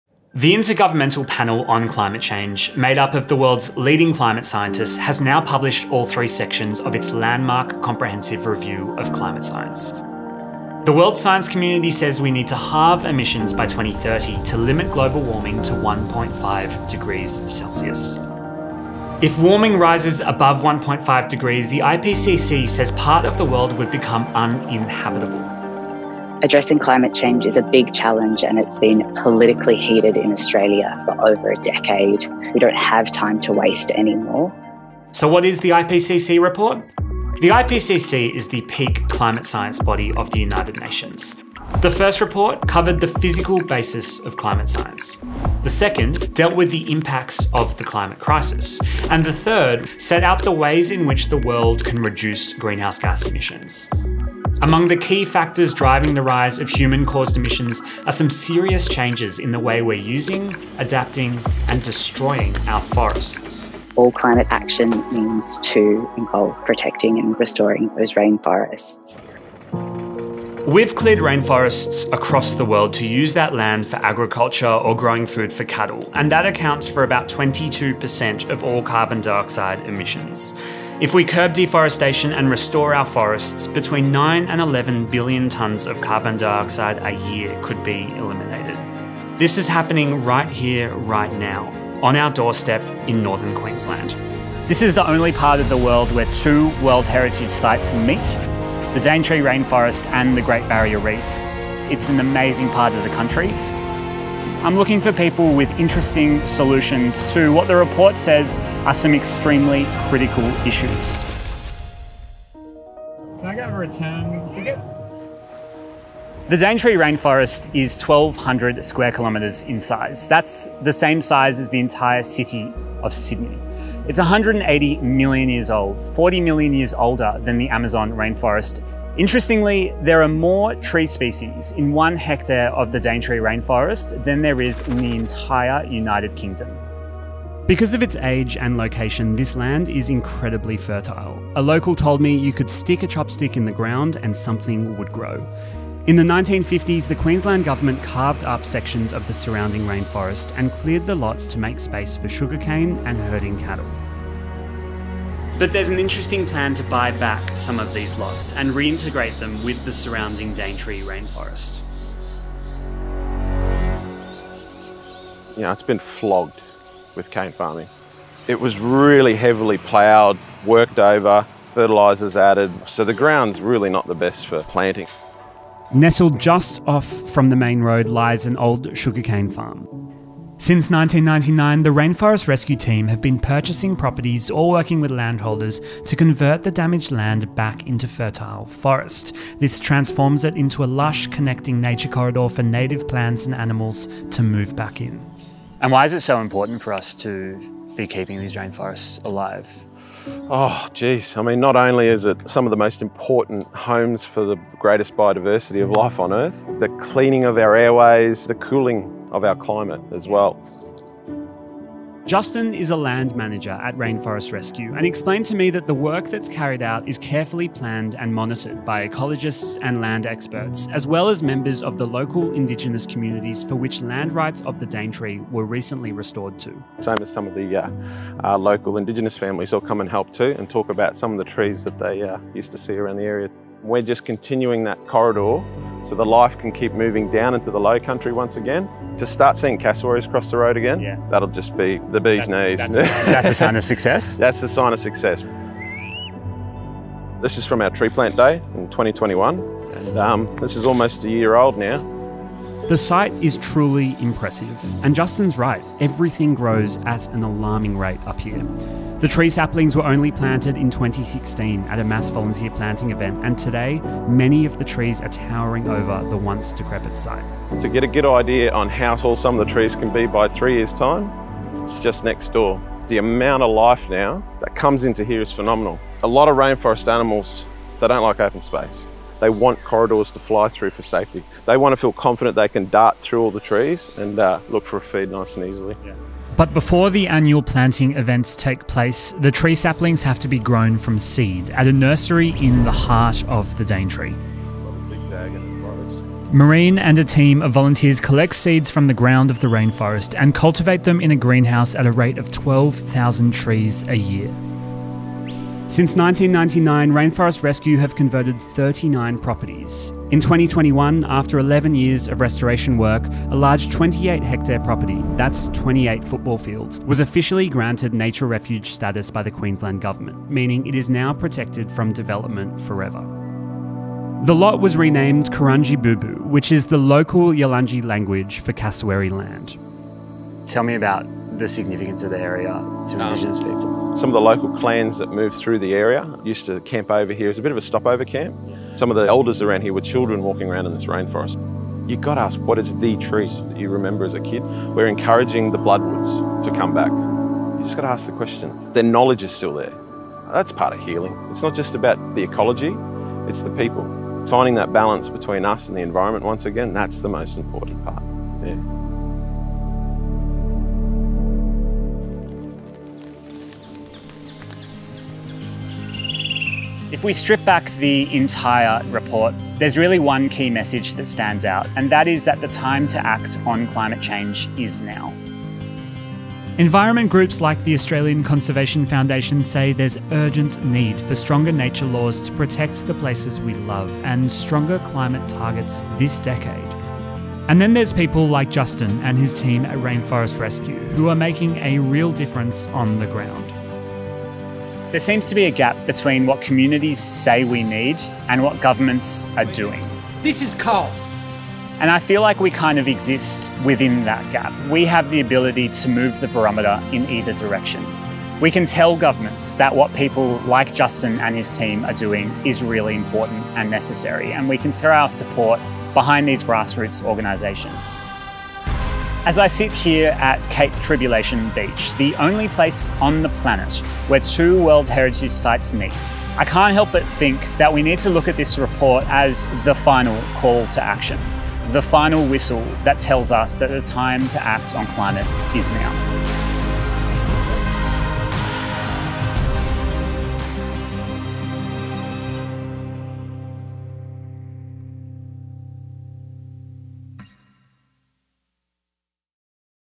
How To Save The World's Oldest Rainforest | The Daily Aus
The IPCC report said it was ‘now or never’ for us to take meaningful climate action. We went to the Daintree Rainforest to see the impacts of climate change for ourselves. Thanks to our partners @australianconservationfoundation for their support on this project, and @rainforestrescue for their work in North Queensland.

Video Chapters:
00:00 - The IPCC Report
01:09 - Deforestation
01:52 - The Daintree Rainforest
02:44 - Rainforest Rescue
04:12 - Re-growing a forest
06:06 - Sam's take
06:59 - Outro